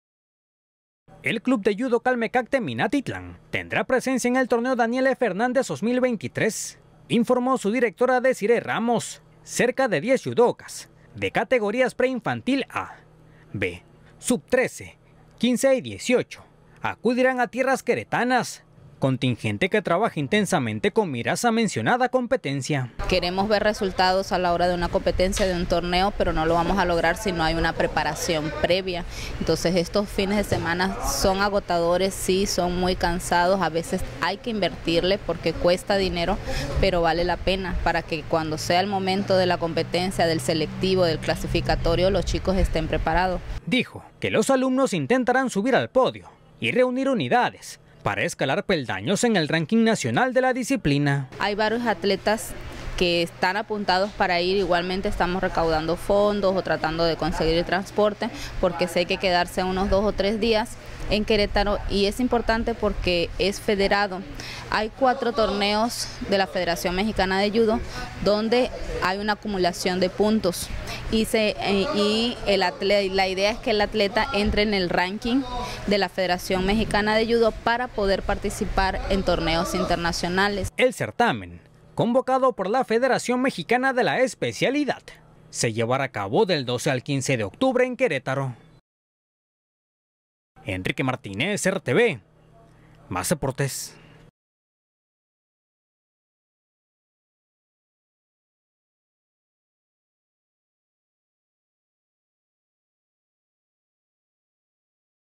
CALMECAC MINATITLÁN A TORNEO
El Club de Judo Calmecac de Minatitlán, participará en el torneo "Daniel Hernández" a realizarse en querétaro en el mes de Octubre.

Reportero: Enrique Martínez